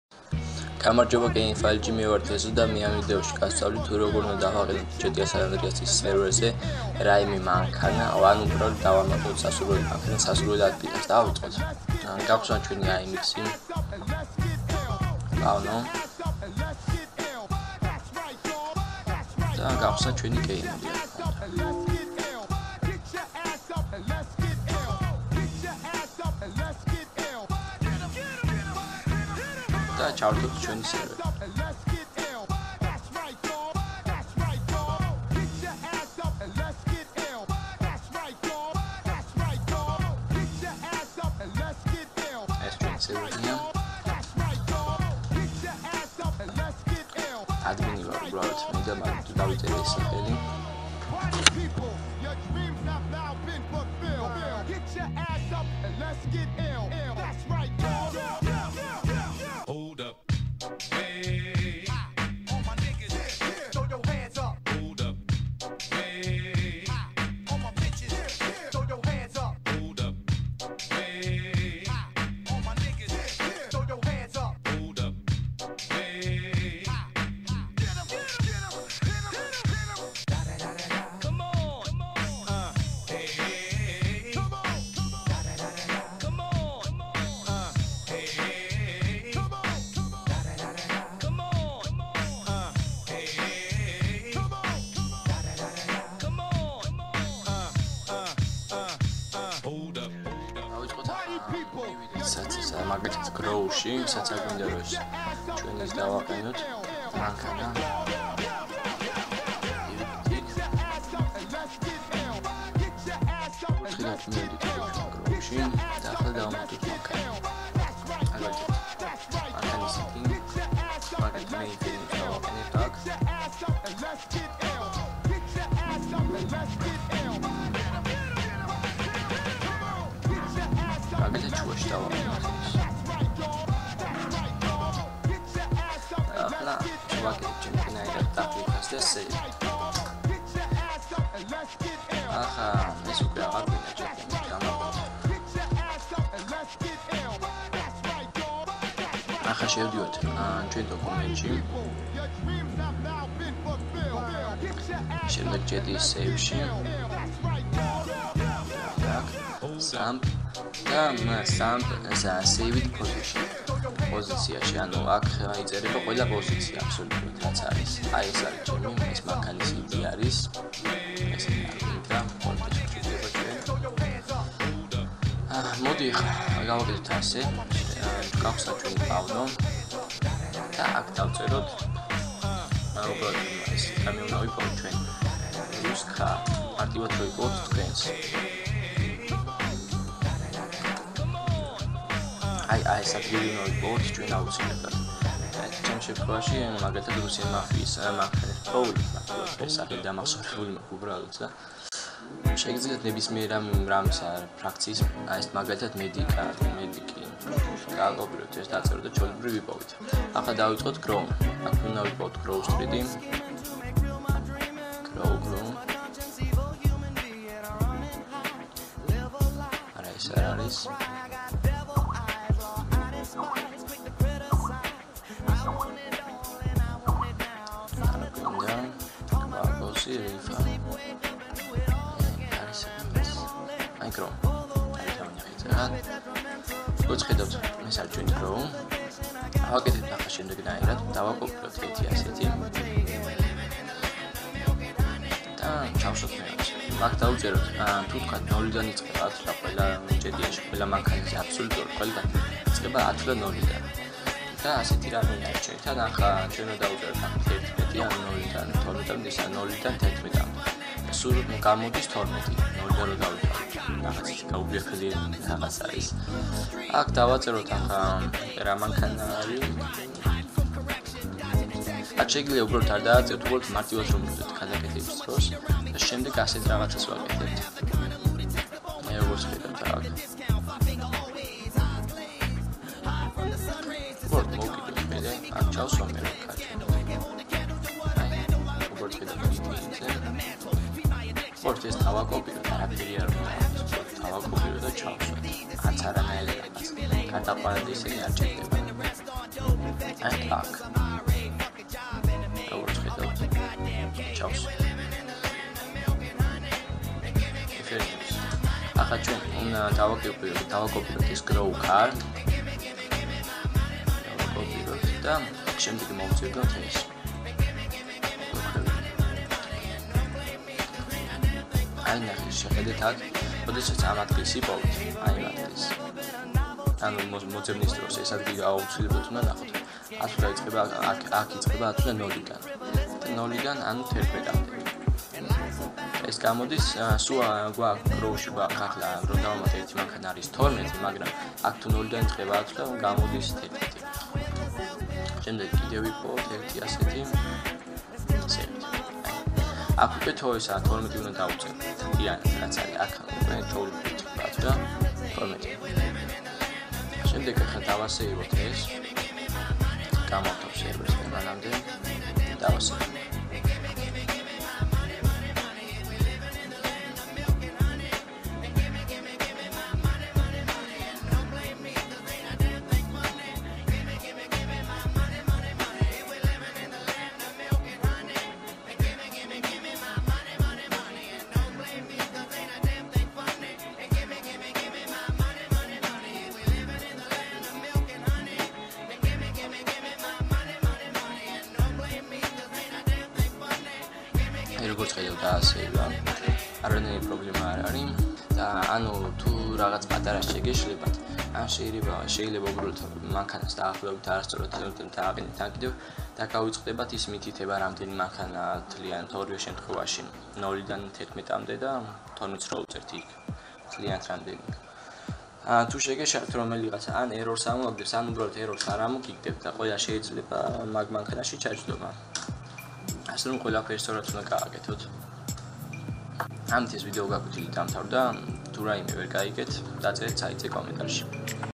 მანქანის დამატება ფრაქციაში - GameFILE.Ge
ამ ვიდეოში ისწავლით მანქანის დამატებას ფრაქციაში ან ბანდში ისე რომ მხოლოდ ვინც ფრაქციაში არის მხოლოდ მათ შეეძლოთ ჩაჯდომა